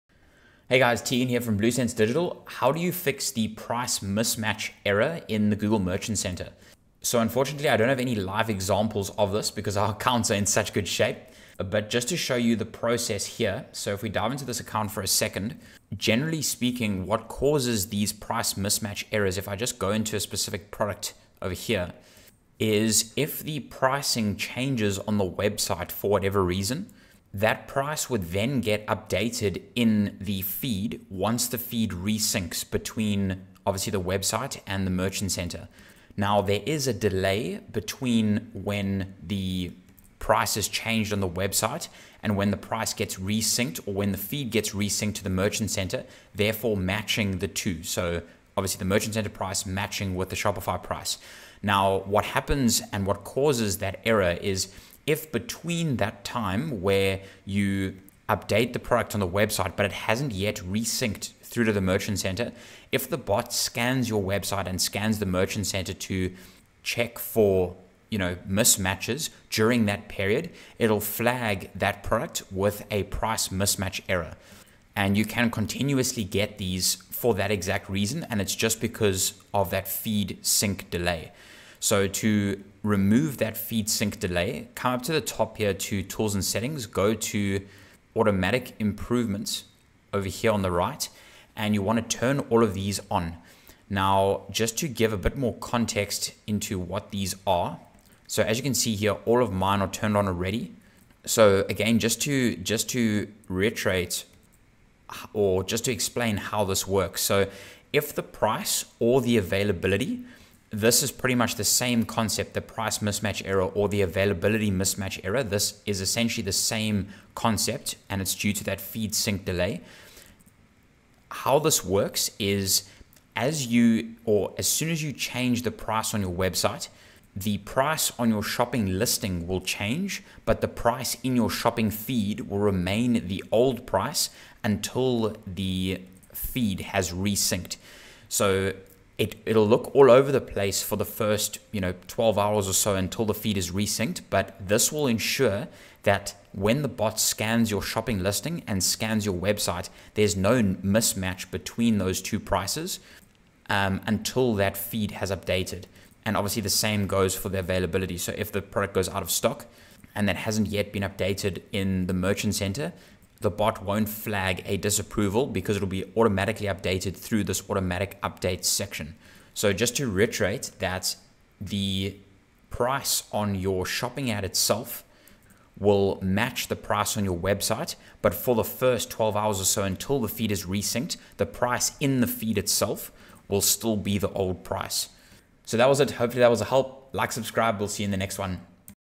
Mismatched value (page crawl): [price] | Price Mismatch Error Fix | Google Merchant Center
In this video, Teagan from Blue Sense Digital goes through how to fix the Mismatched value (page crawl): [price] or commonly referred to the Price Mismatch Error in the Google Merchant Center. Hopefully this is a help :)